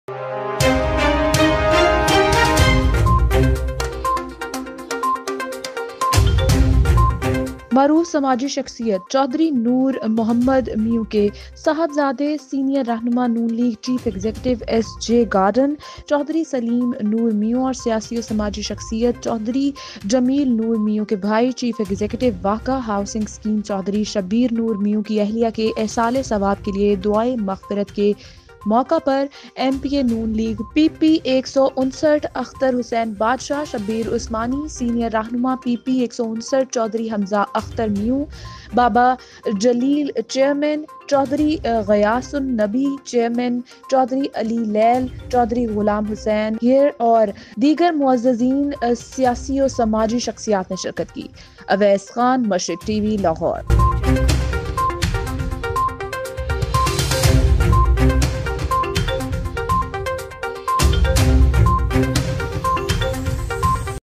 چیف ایگزیکٹیو واہگہ ہاوسنگ سکیم چوہدری شبیر نور میئو کی اہلیہ کے ایصال ثواب کیلئے دعائے مغفرت
اویس خان لاہور